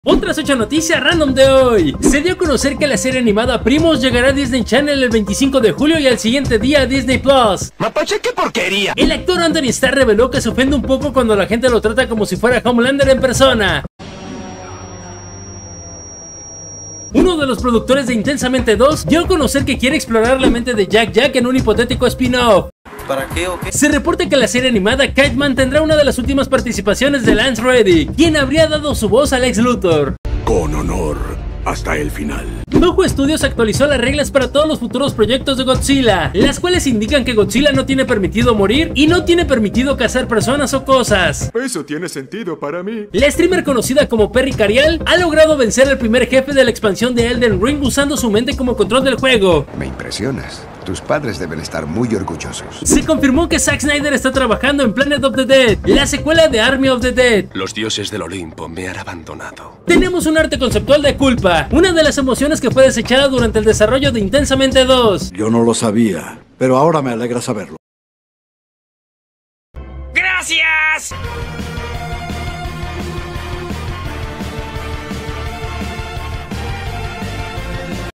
Estreno de PRIMOS, nuevas reglas para GODZILLA, emoción CULPA, PLANET OF THE DEAD, KITE-MAN y MÁS
Otras 8 noticias random

noticias ultimahora videojuegos películas cine otrasnoticiasrandom noticiasrandom